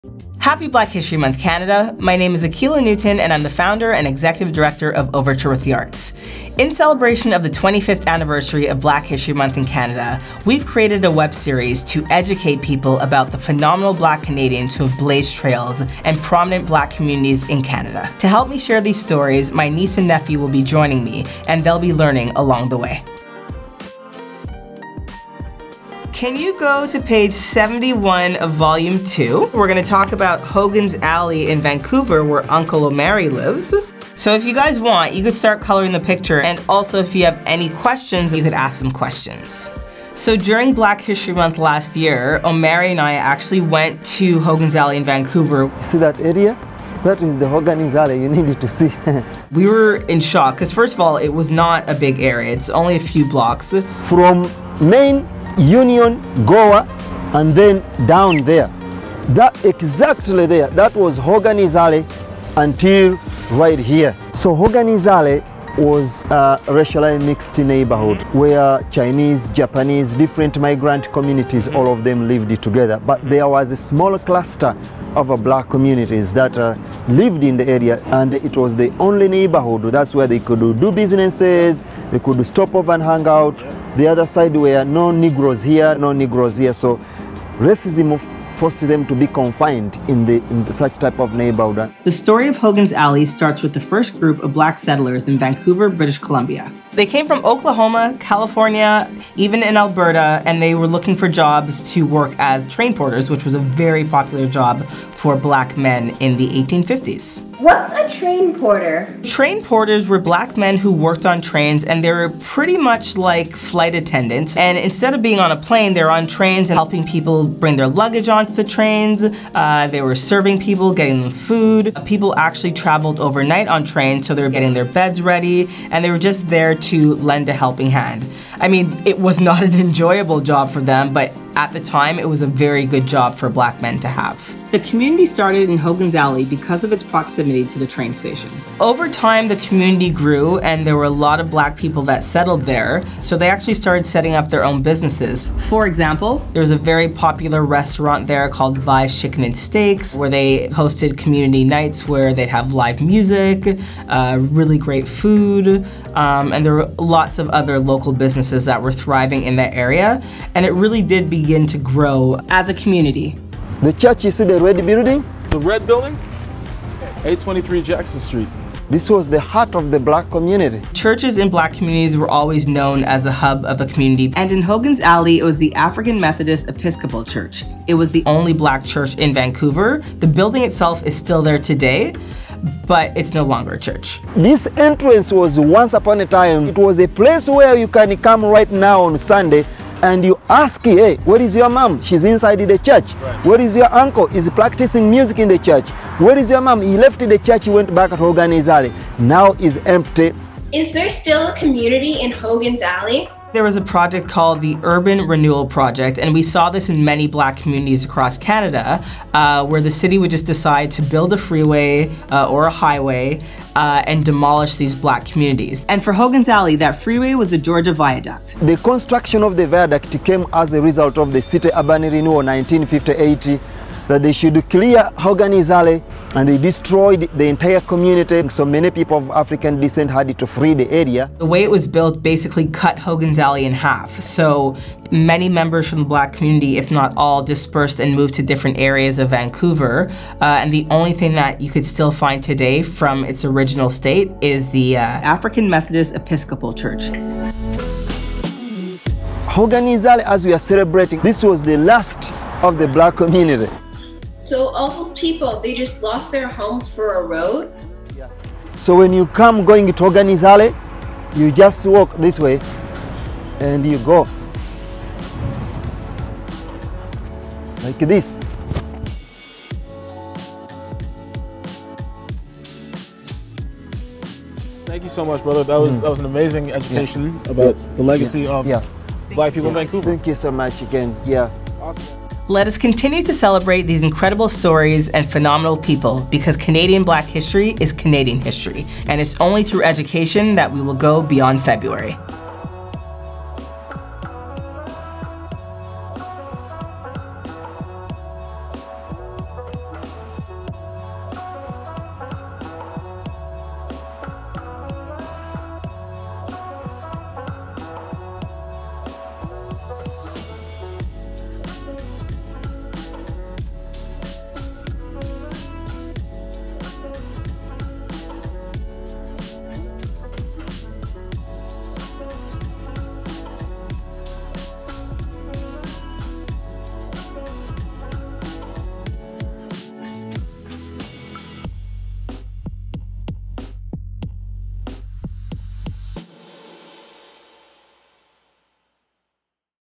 BEYOND FEBRUARY | Episode 1: Hogan's Alley, Vancouver Black History
The first video in the Beyond February series will highlight Hogan’s Alley, a once thriving Black community in Vancouver, British Columbia.